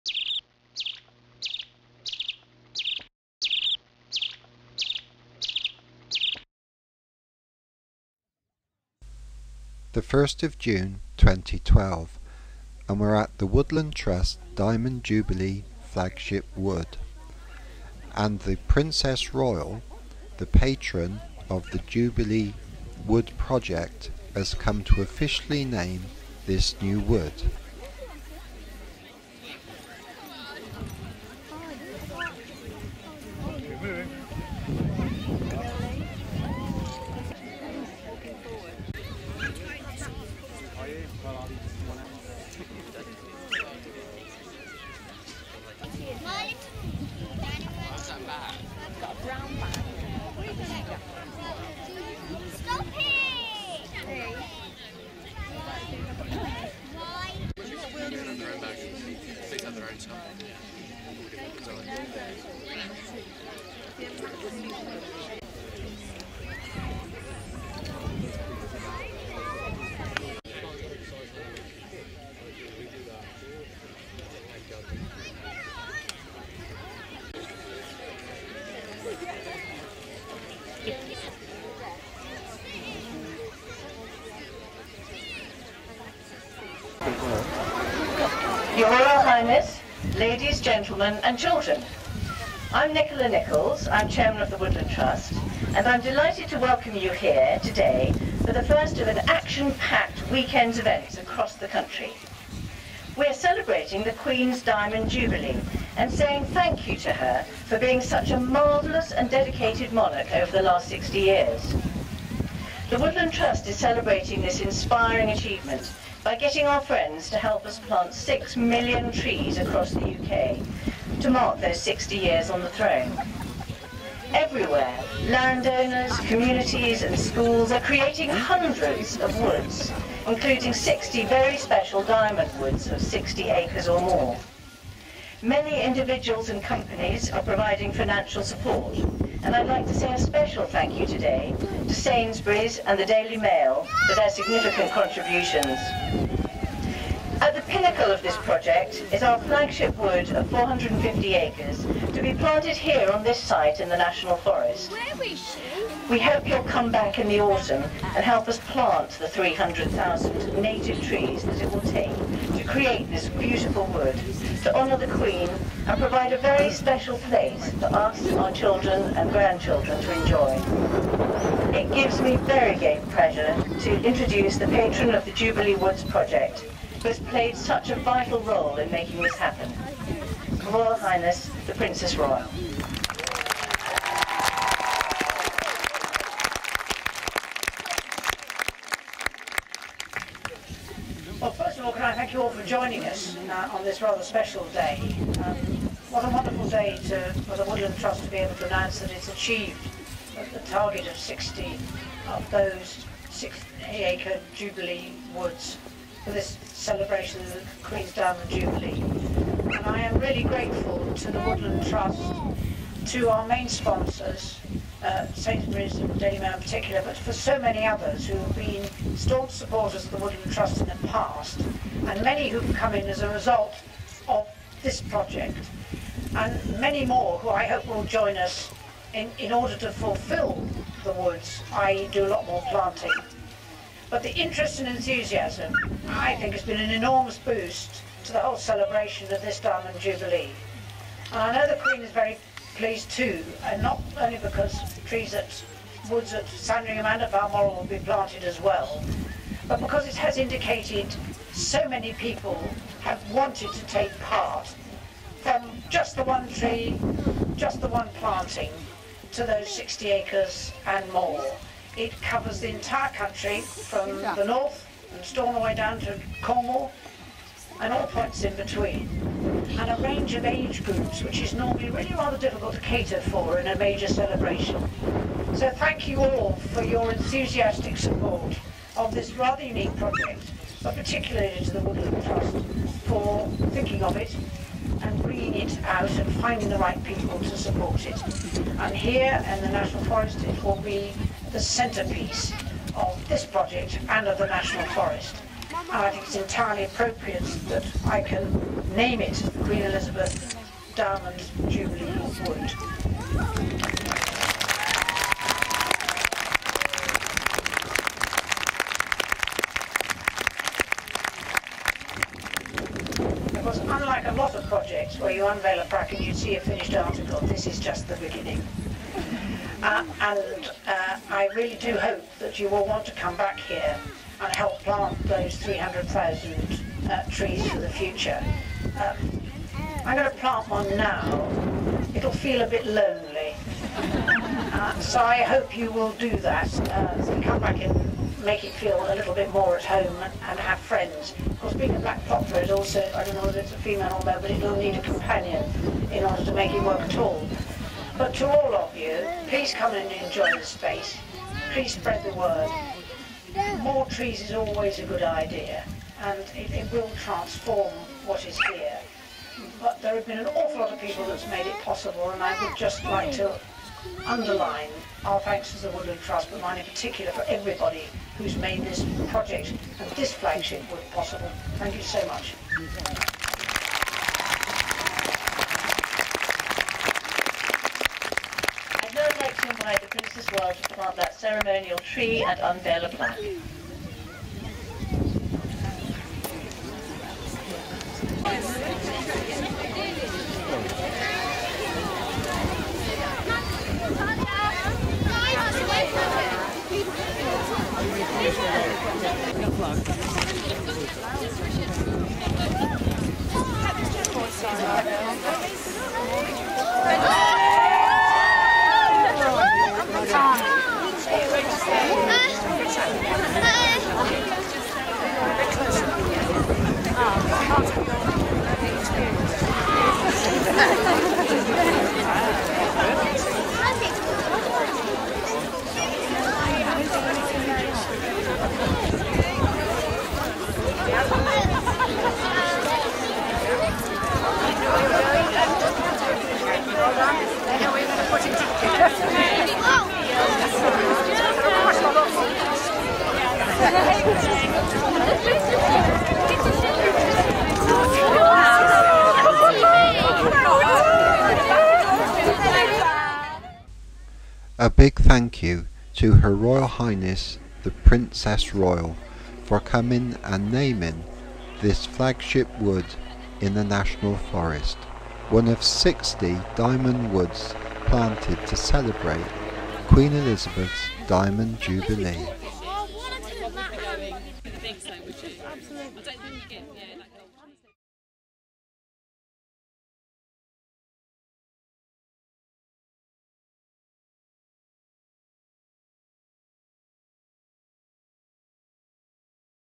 The Diamond Jublee Flagship Wood The Princess Royal's visit
The Diamond Jublee Flagship Wood in the National Forest at the heart of England visit by Her Royal Highness The Princess Royal on 1 June 2012.